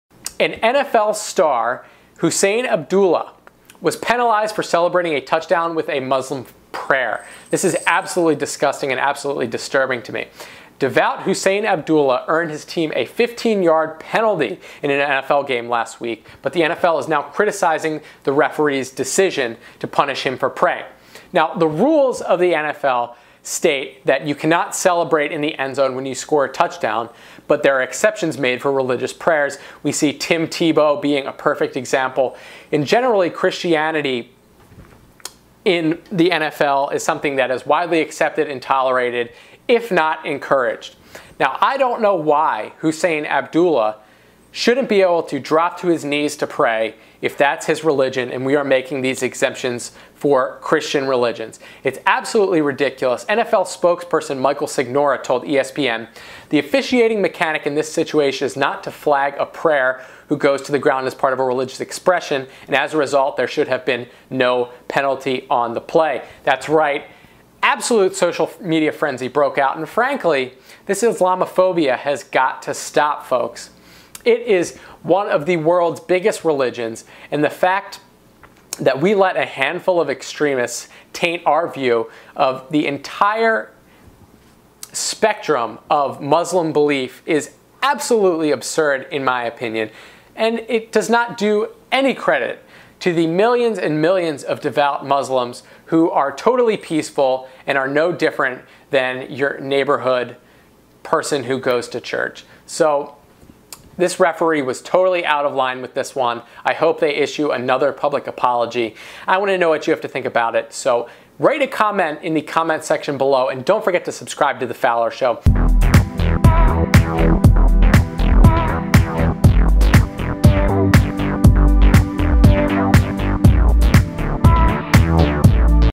Husain Abdullah Penalised For Celebrating With Muslim prayer
Devout Husain Abdullah, earned his team a 15-yard penalty, but the NFL has criticized the referee's decision to punish him for praying

An American football star was penalised for "unsportsmanlike conduct" after he celebrated a touchdown by performing a Muslim prayer.

Devout Husain Abdullah, 29, was slapped with a 15-yard penalty when he dropped to his knees to pray after scoring for Kansas City Chiefs in their 41-14 win over New England Patriots on Monday.

Under National Football League (NFL) rules, players are banned from celebrating or demonstrating while on the ground, but exceptions are made for religious prayers.

However, the NFL said the referee made the wrong judgement.

Muslim NFL player penalised for prayer
Punished: Muslim NFL star Husain Abdullah was penalised for praying
NFL spokesman Michael Signora told ESPN: "The officiating mechanic in this situation is not to flag a player who goes to the ground as part of religious expression, and as a result, there should have been no penalty on the play."

The penalty sparked a social media frenzy among American football fans, who said Christian players have been allowed to pray in similar situations.

Abdullah took to Twitter to thanked fans for their support.

In 2012, Abdullah, along with brother and fellow NFL star Hamza, was absent throughout the 2012 season while on a pilgrimage to Mecca.

3 Steps To Join FowlerNation!
2. 'Like' The Richard Fowler Show on Facebook
3. 'Follow' Us on Twitter

Truthservers.com